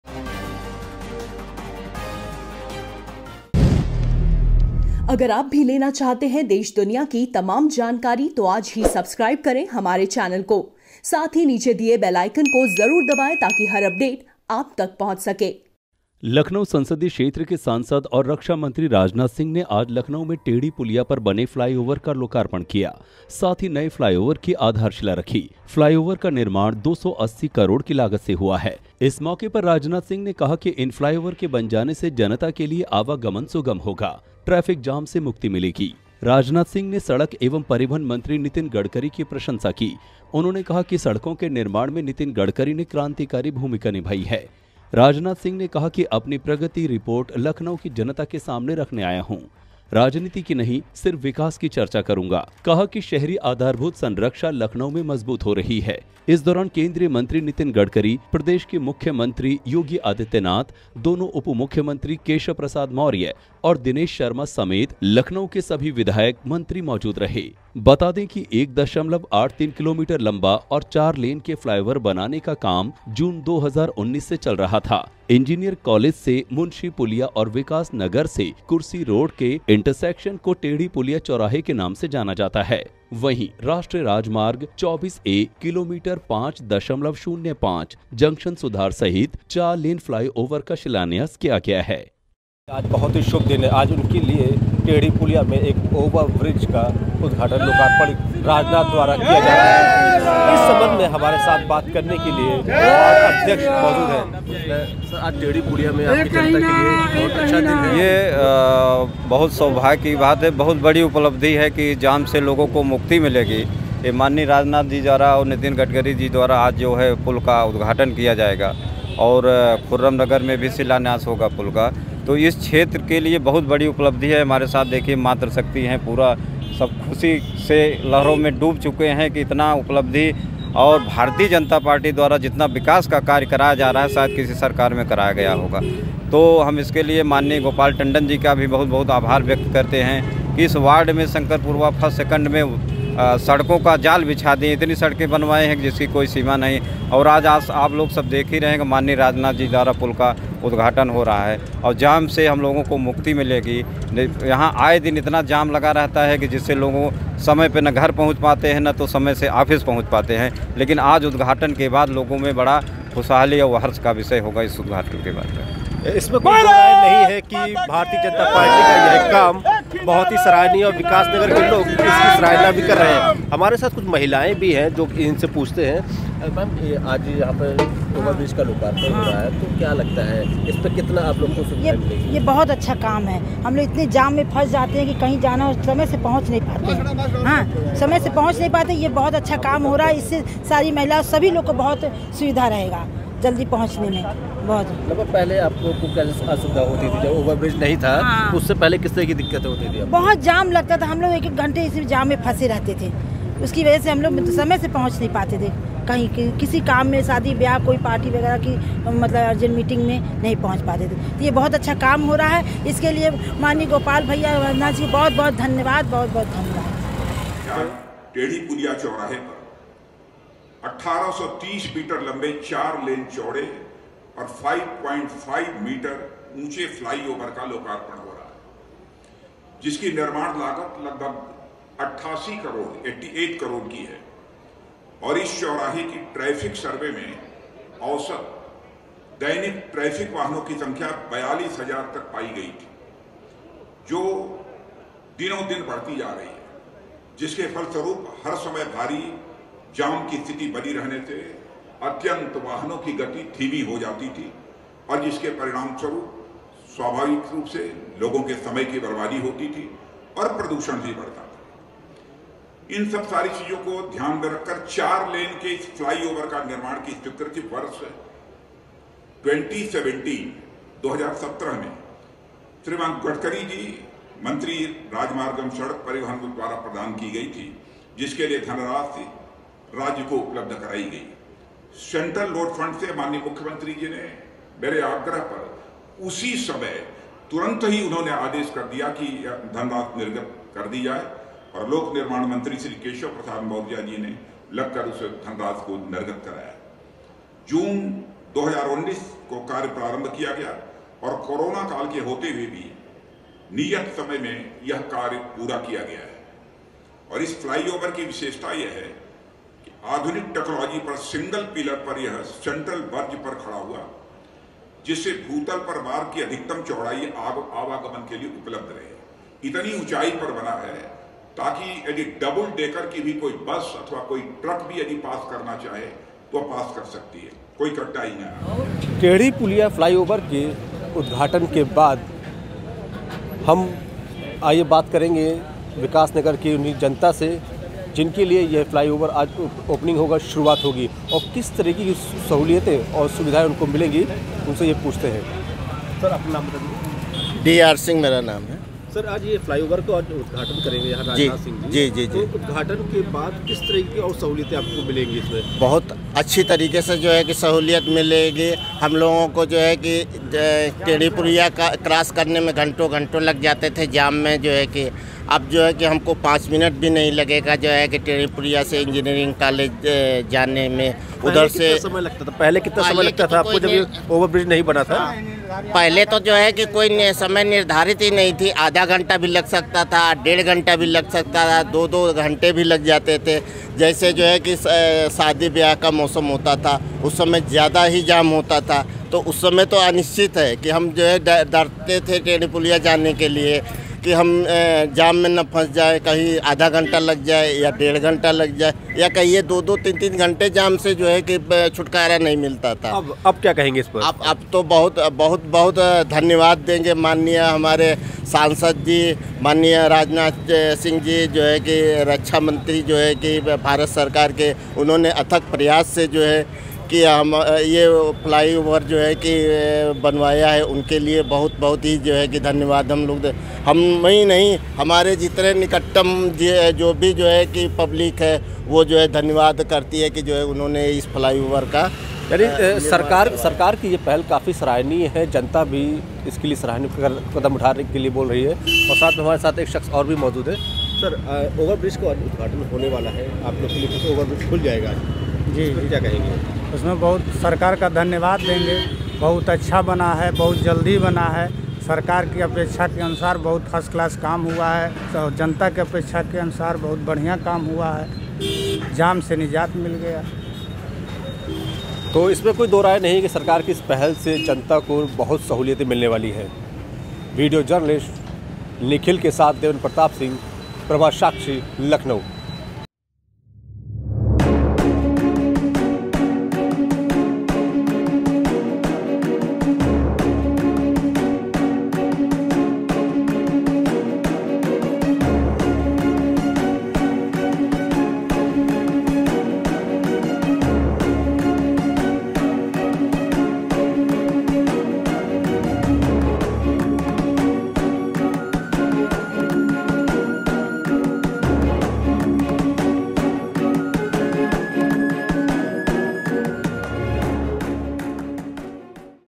लखनऊ में टेढ़ी पुलिया फ्लाईओवर का लोकार्पण और खुर्रम नगर फ्लाईओवर का शिलान्यास
लखनऊ संसदीय क्षेत्र के सांसद और रक्षा मंत्री राजनाथ सिंह ने शुक्रवार को लखनऊ में टेढ़ी पुलिया पर बने फ्लाईओवर का लोकार्पण किया। साथ ही खुर्रम नगर के नए फ्लाईओवर की आधारशिला रखी। फ्लाईओवर का निर्माण 280 करोड़ की लागत से हुआ है। इस मौके पर राजनाथ सिंह ने कहा कि इन फ्लाईओवर के बन जाने से जनता के लिए आवागमन सुगम होगा और ट्रैफिक जाम से मुक्ति मिलेगी। राजनाथ सिंह ने सड़क एवं परिवहन मंत्री नितिन गडकरी की प्रशंसा की। उन्होंने कहा कि सड़कों के निर्माण में नितिन गडकरी ने क्रांतिकारी भूमिका निभाई है। राजनाथ सिंह ने कहा कि अपनी प्रगति रिपोर्ट लखनऊ की जनता के सामने रखने आया हूं। राजनीति की नहीं सिर्फ विकास की चर्चा करूंगा। उन्होंने कहा कि शहरी आधारभूत संरक्षा लखनऊ में मजबूत हो रही है।

इस दौरान केंद्रीय मंत्री नितिन गडकरी, उत्तर प्रदेश के मुख्यमंत्री योगी आदित्यनाथ, दोनों उप मुख्यमंत्री केशव प्रसाद मौर्य और दिनेश शर्मा समेत लखनऊ के सभी विधायक और मंत्री मौजूद रहे। बता दें कि 1.83 किमी लंबे और चार लेन के फ्लाईओवर को बनाने का काम जून 2019 से चल रहा था। इंजीनियर कॉलेज से मुंशी पुलिया और विकास नगर से कुर्सी रोड के इंटरसेक्शन को टेढ़ी पुलिया चौराहे के नाम से जाना जाता है। वहीं, राष्ट्रीय राजमार्ग 24A किलोमीटर 5.05 (खुर्रम नगर) जंक्शन सुधार सहित चार लेन फ्लाई ओवर का शिलान्यास किया गया है।